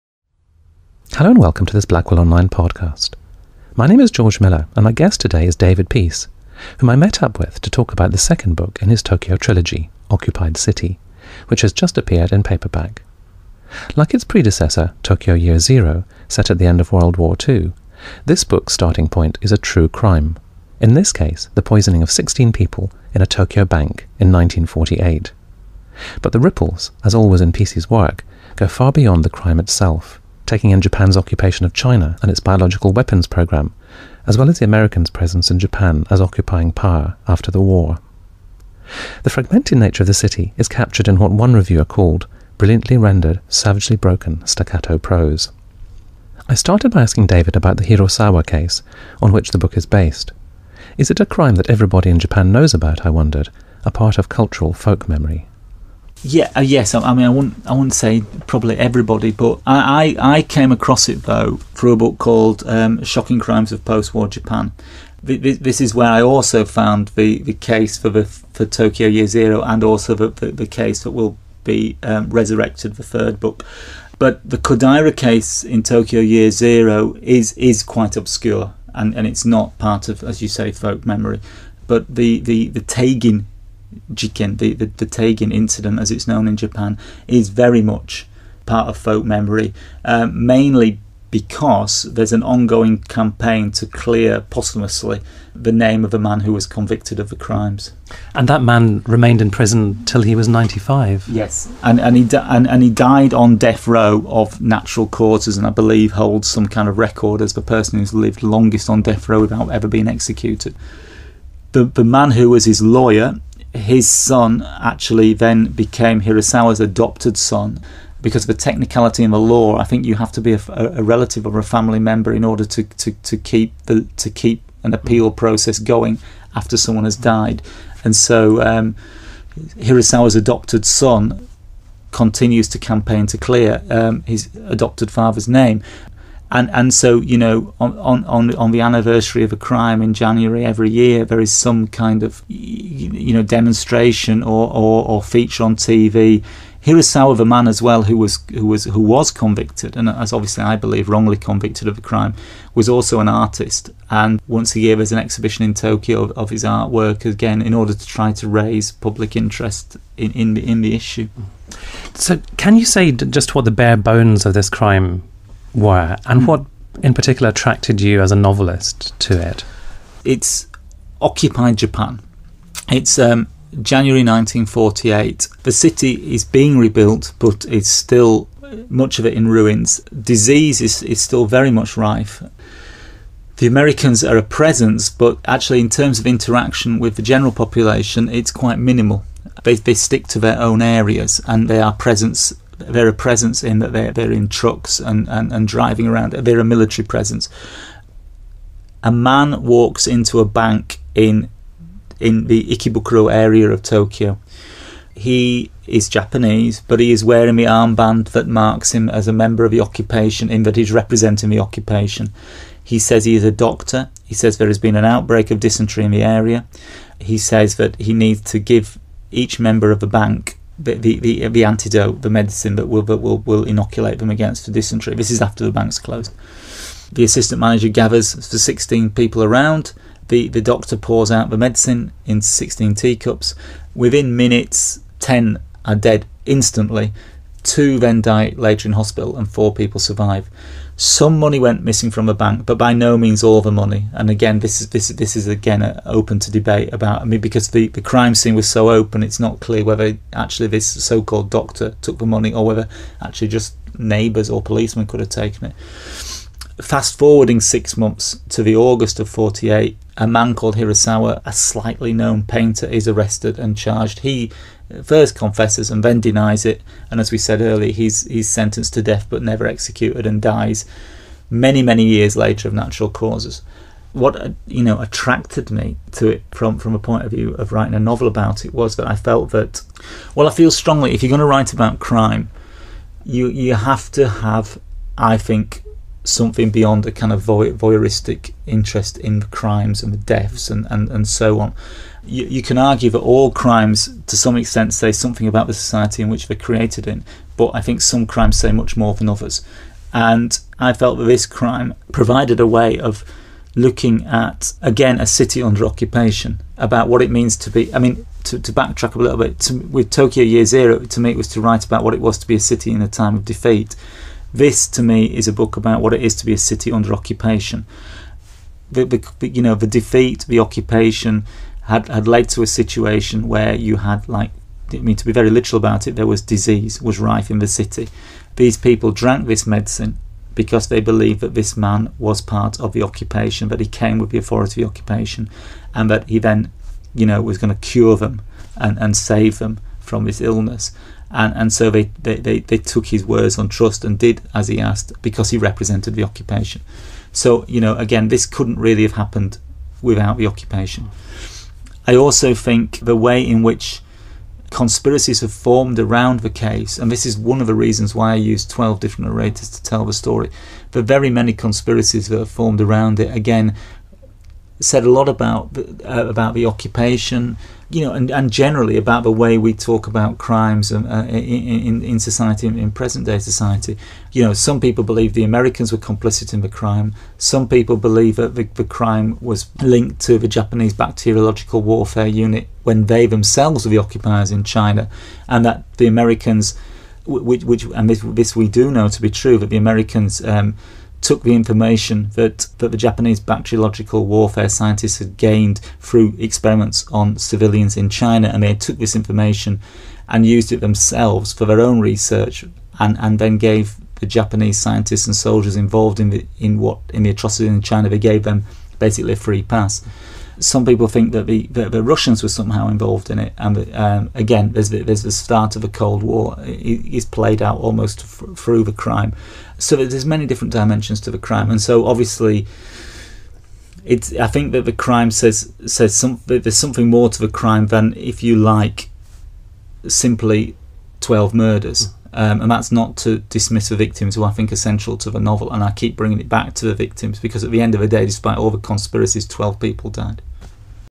David Peace - Occupied City - Part 1 of 2
David Peace is the author of 'The Red Riding Quartet', 'The Damned Utd' and new in paperback, 'Occupied City'; the second book in his 'Tokyo Trilogy'. The book dramatises the notorious real-life case of Japan's Teikoku Bank Massacre that took place in 1948, exploring the man who was convicted of the crime, the victims, survivors and more.